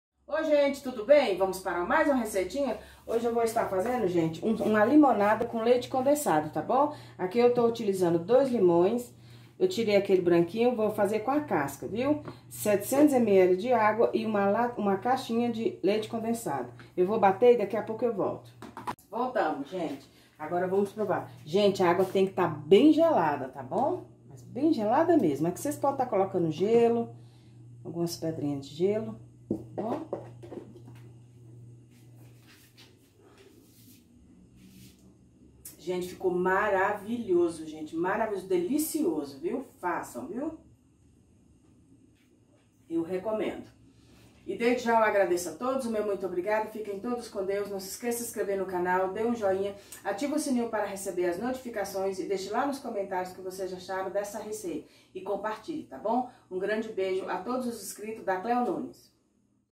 Limonada com leite condensado
Ingredientes:
2 limões
700ml de água
1 caixinha de leite condensado